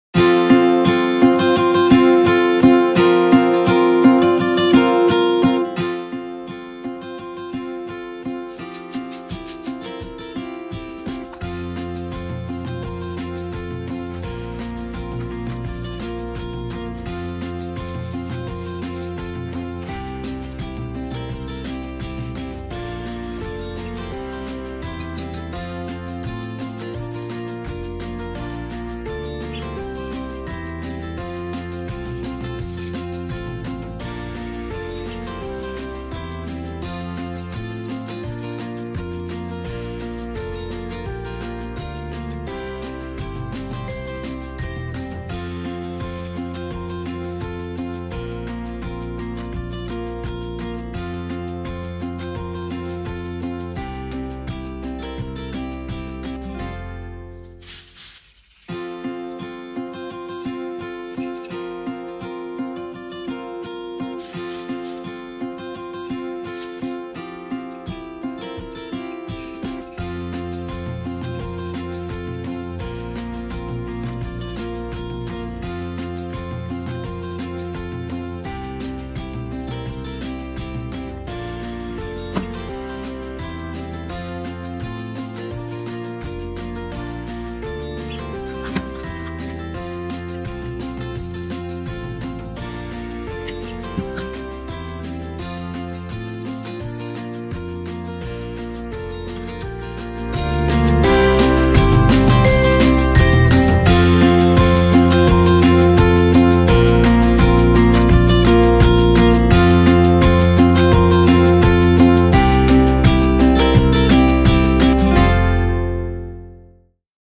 Awesome eco-friendly way to reuse old glass utensils
This is an instructive video about the preparation of a simple glass jar decor plant which will be placed on my desk in our new office. The plants are Ficus Benjamina, the soil is regular potting soil and the clay pebbles are washed well. The glass jar does not need holes at the bottom if you don't flood the soil. Roots need some air but if you just keep the soil moist and not excessively wet, they will get enough fresh air by diffusion. Have fun and make the world greener! :) Soon I will hopefully make an artificial lighting apparatus to boost their growth through the winter.